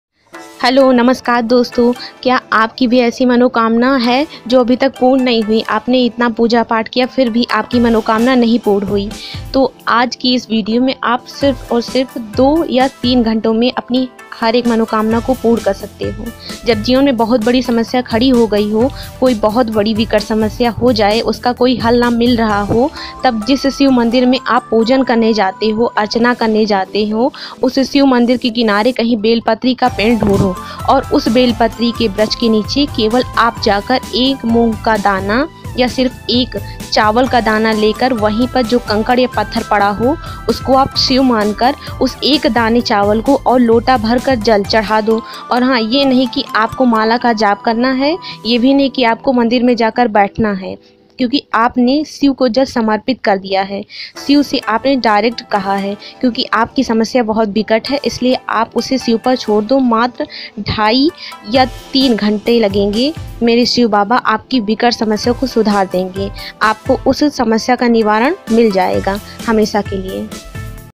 मनोकामना पूर्ति करने के अचूक उपाय | शिवपुराण की कथा | अधिक पूजा करने के बाद भी मनोकामना पूर्ण न हो
तो क्या करे और जाने आखिर क्यों हमारी मनोकामना पूर्ण नही होती है।
मनोकामना पूर्ति करने के उपाय है।